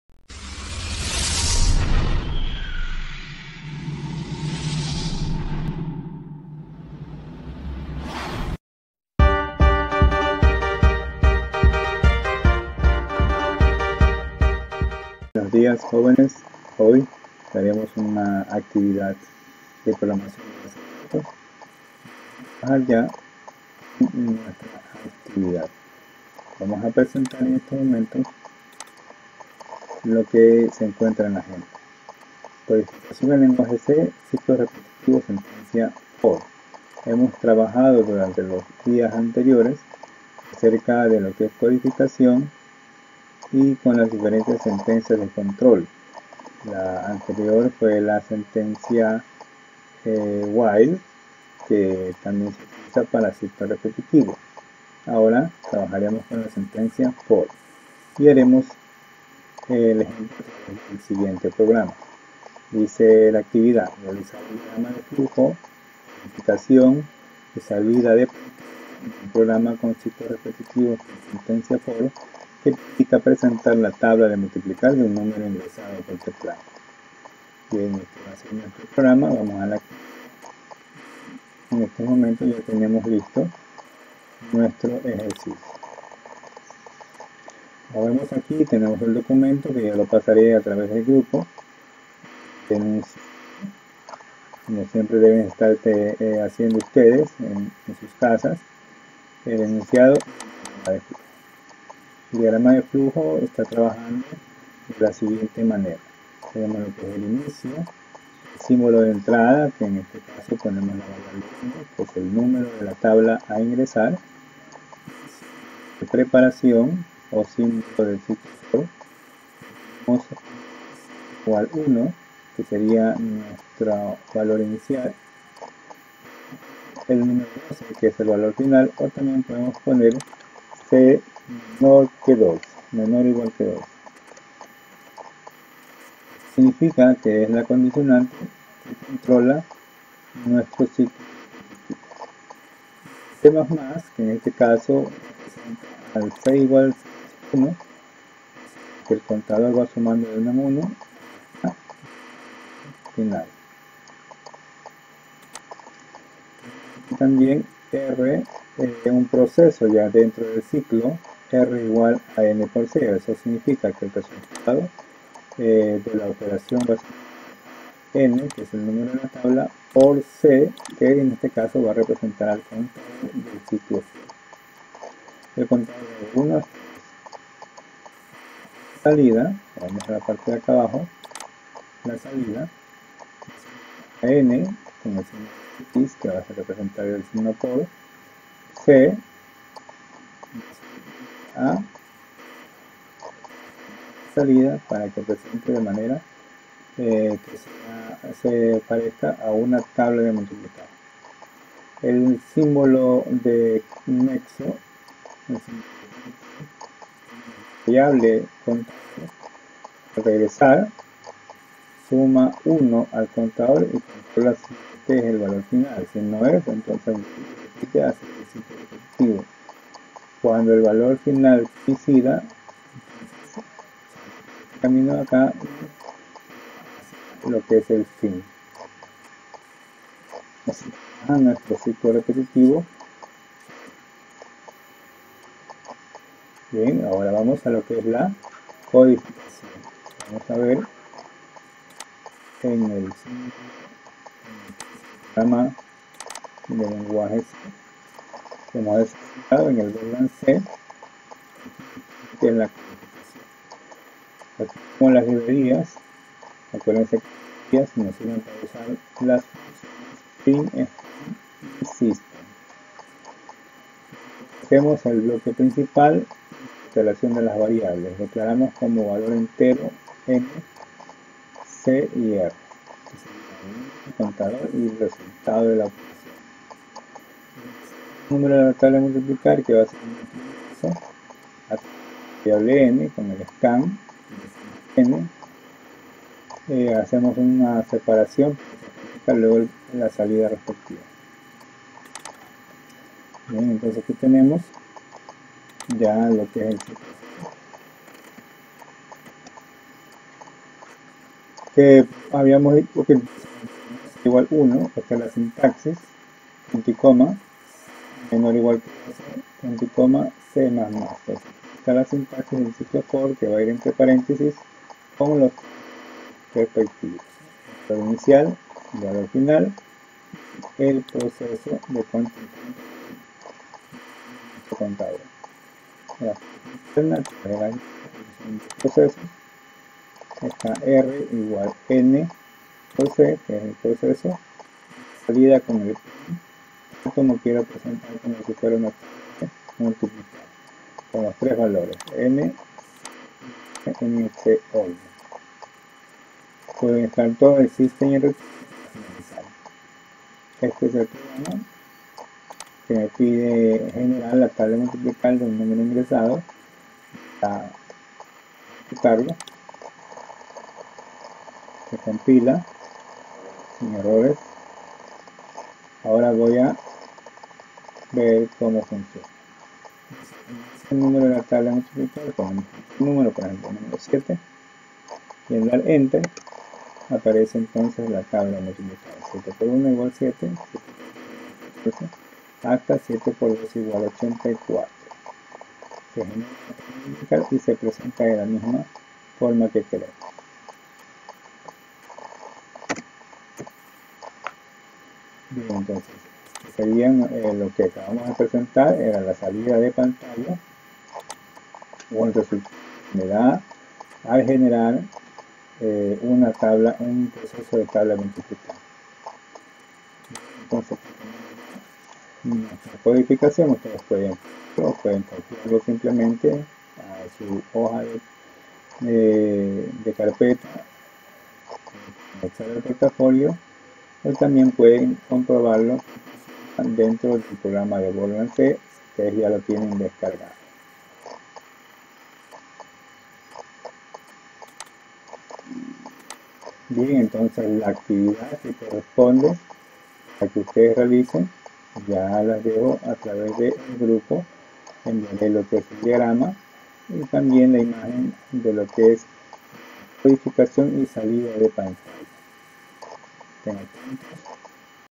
Codificación en Lenguaje C: Ciclo For, Ejemplo
Clase de Programación y BD para Primero de bachillerato Perfil Informática Semana 37
(fallas de origen en Audio)